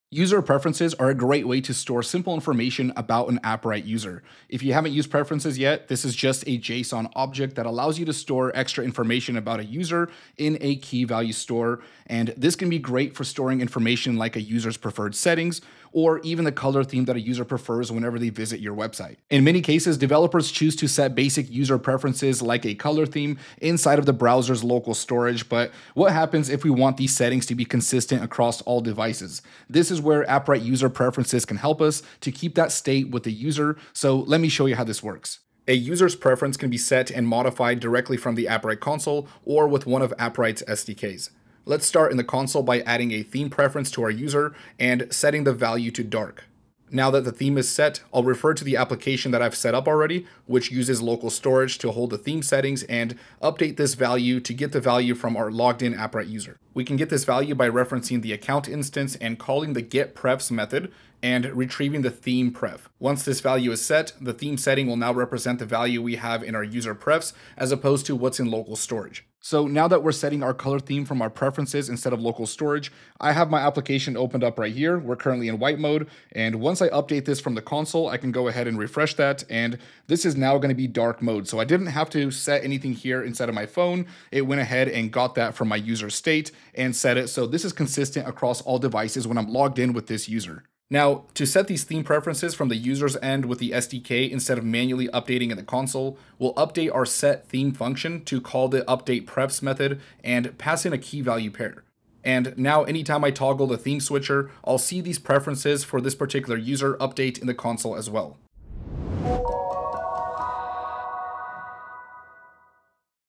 Adding custom user data with Preferences
User preferences provide a quick and simple solution for adding custom about an Appwrite user without the need for us to modify the database schema. User Preferences are a JSON object that can store up to 64 KB of user-related information.